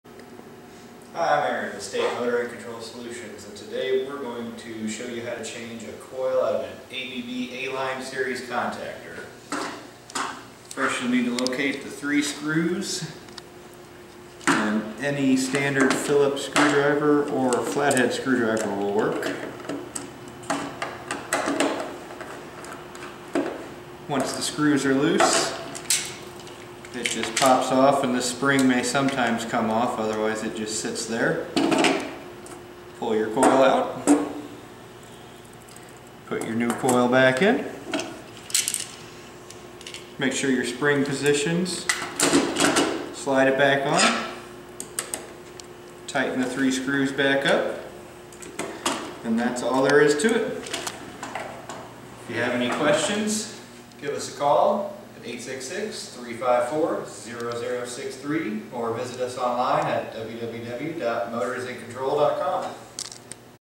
ABB A Line Contactor Coil Change
Changing the coil in an ABB A Line Motor Starter Contactor is a simple procedure.  Watch as Aaron from State Motor & Control solutions show you how.